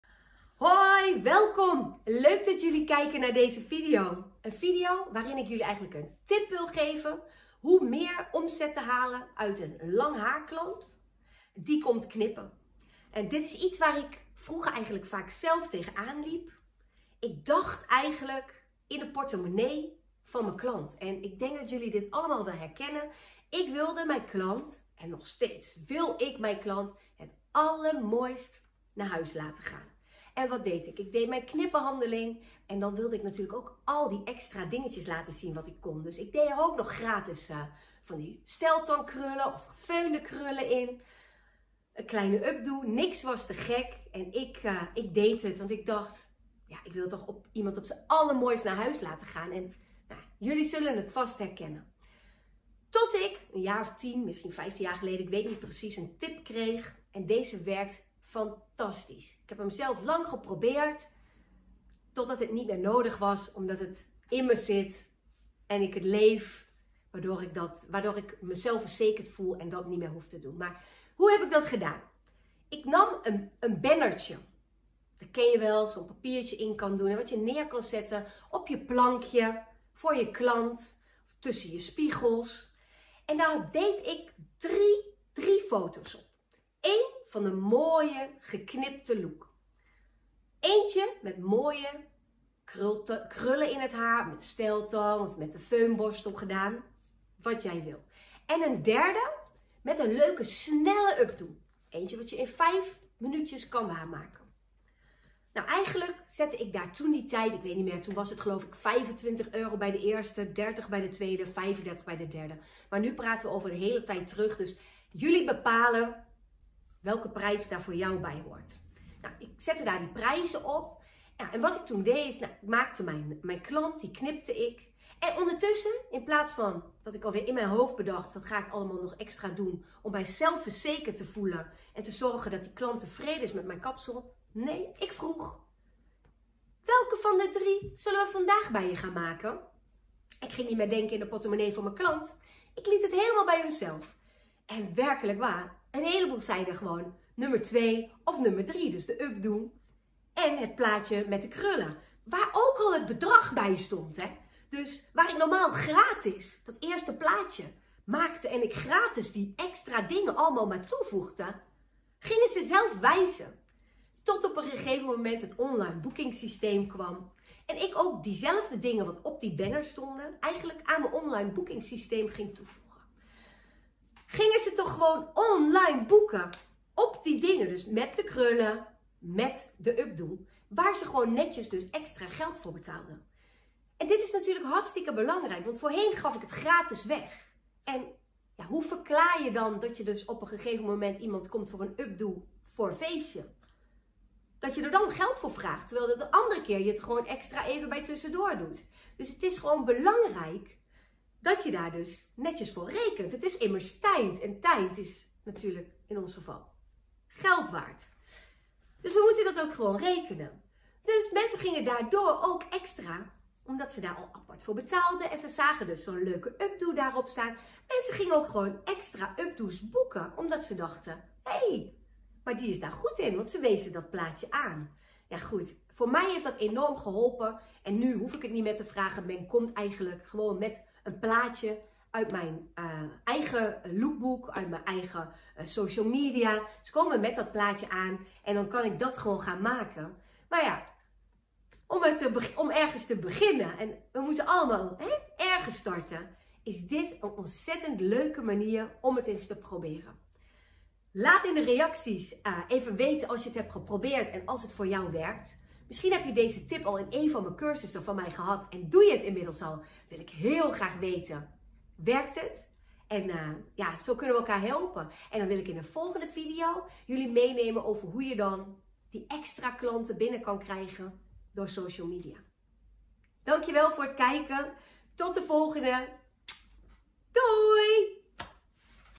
Tip als je gratis krullen, half updo,s en vlechten weggeeft na je behandeling.
Hoi kappers vrienden.

In deze video geef ik jullie een tip waarmee je de omzet uit een klant die een knip of kleur behandeling bij jouw boekt kan verhogen.

Wanneer je dit een periode gaat doen ga je uiteindelijk veel zelfvertrouwen ontwikkelen omdat je ziet dat het werkt en omdat je vaker een updo gaat maken.

Neem een banner en zet daar een papier in met  3 foto’s erop.
1 van normaal geknipt haar.
1 van de krullen die je normaal gratis weggeeft.
1 van een snelle updo.
Zet er een prijs bij.
Bij foto 1-2 en 3 een steeds hogere prijs.
Een bedrag waar jij je goed bij voelt.

Vraag de klant 1 van de foto’s te kiezen.

Nu hebben ze de prijs gezien en hoef je je bij de kassa niet meer ongemakkelijk te voelen.

Laat je mij in de reacties weten wat je van deze tip vindt?

Succes
Groetjes Soraya